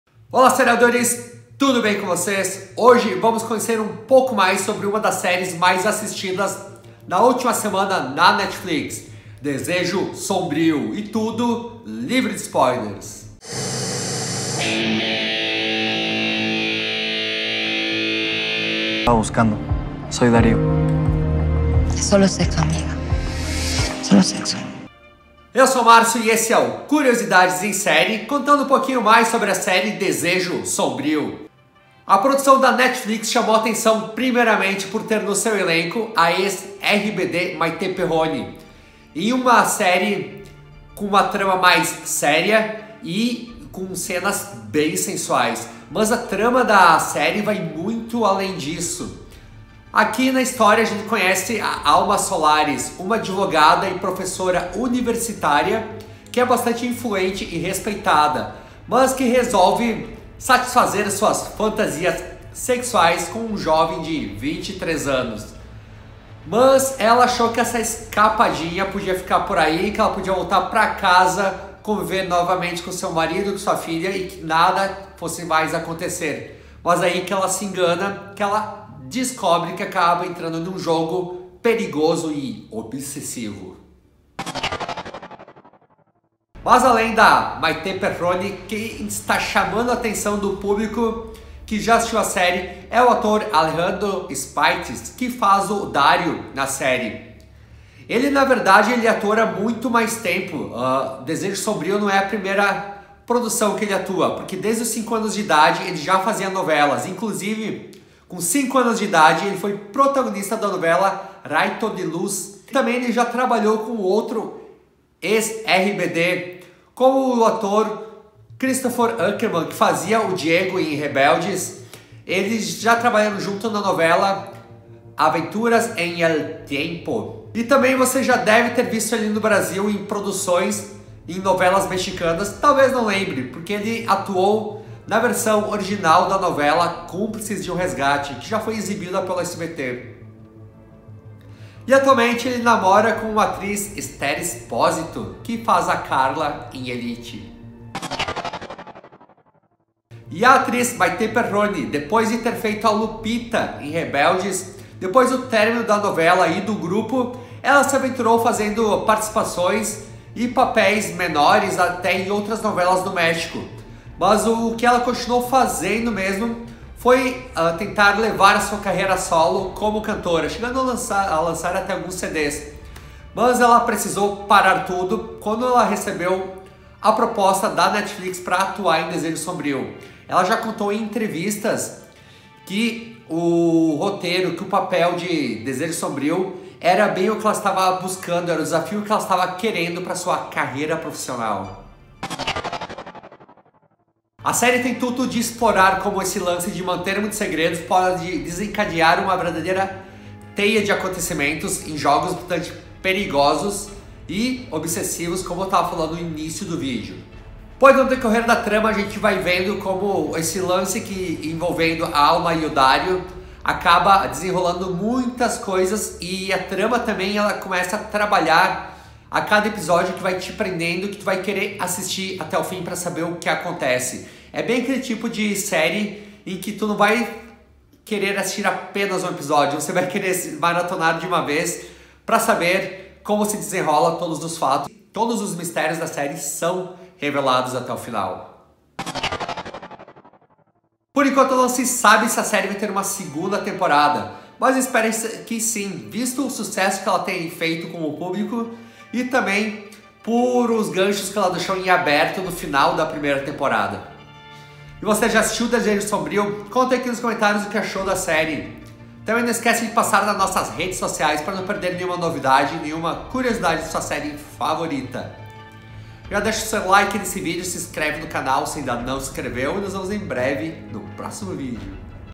CONHEÇA MAIS SOBRE DESEJO SOMBRIO
Muito mais que a presença da ex-RBD Maite Perroni no elenco, conheça um pouco mais sobre Desejo Sombrio, uma série envolvente e surpreendente.

▶ Tem alguma opinião sobre o vídeo de hoje? Ou uma sugestão legal de tema para o próximo? Deixe nos comentários!

Roteiro e edição: Márcio de Oliveira